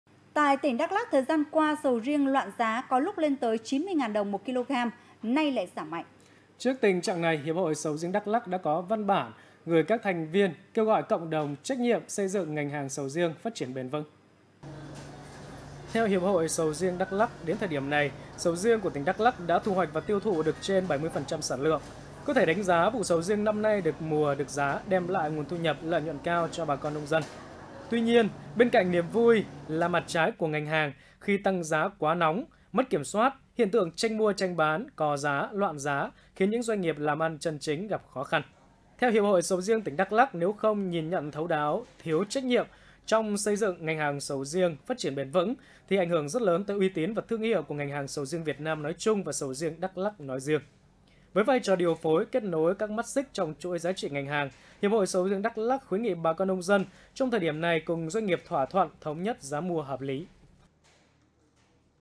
Tìm giải pháp xây dựng ngành hàng sầu riêng bền vững| Truyền hình Đắk Lắk
Tìm giải pháp xây dựng ngành hàng sầu riêng bền vững#thdaklak

ĐÀI PHÁT THANH TRUYỀN HÌNH ĐẮK LẮK